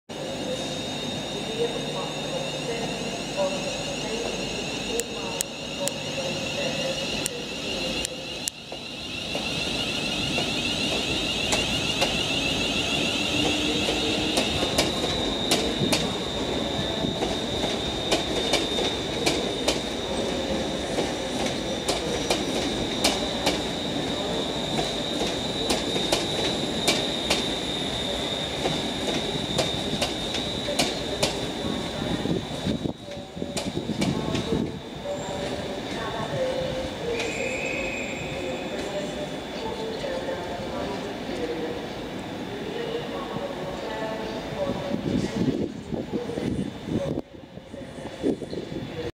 スコットレイル クラス380 グラスゴー中央駅発車 ScotRail Class 380
VVVFの磁励音が独特なスコットレイルのクラス380電車がグラスゴー・セントラル駅9番線から出発する。
British Rail Class 380 train will depart from Glasgow Central station, Scotland.